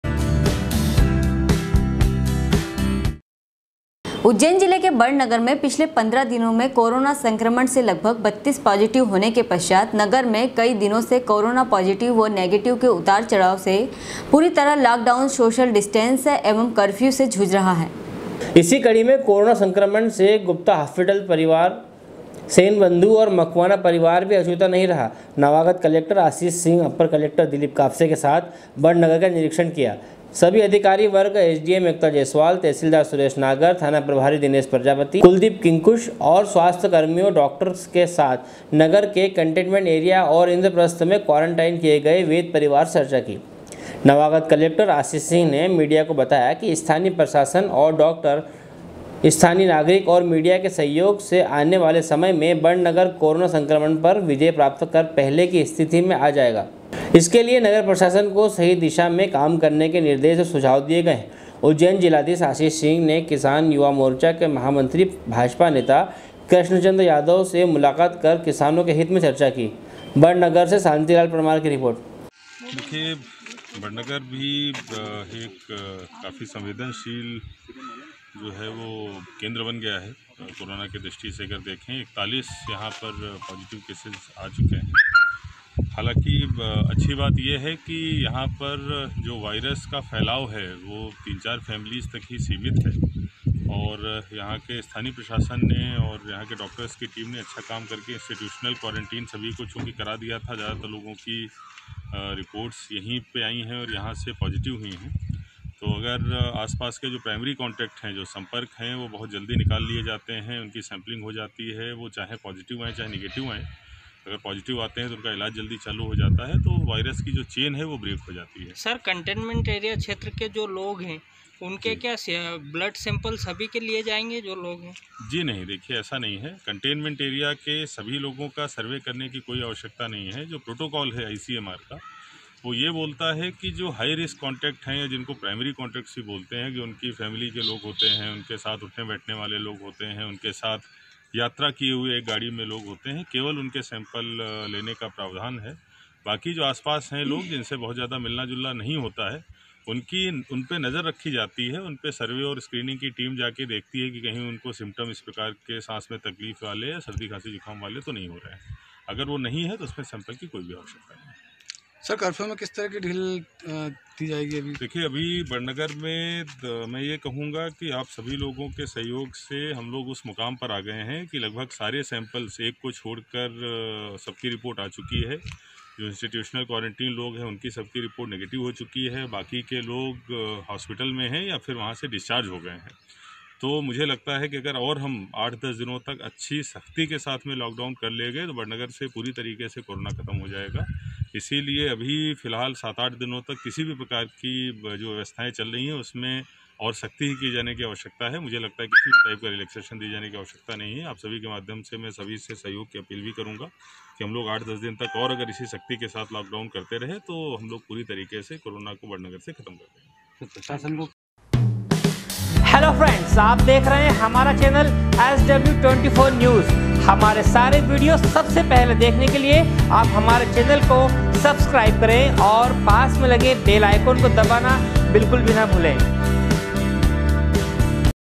SW 24 News Madhyapradesh#badhnagar#09.05.2020
sw sw24news sw24newsmadhyapradesh livenews news24उज्जैन जिले के बड़नगर में पिछले 15 दिनों में कोरोना संक्रमण से लगभग 32 पॉजिटिव होने के पश्चात नगर में कई दिनों से कोरोना पॉजिटिव व नेगेटिव के उतार-चढ़ाव से पूरी तरह लॉक डाउन, सोशल डिस्टेंस एवं कर्फ्यू से जूझ रहा है।
बड़नगर से शांतिलाल परमार की रिपोर्ट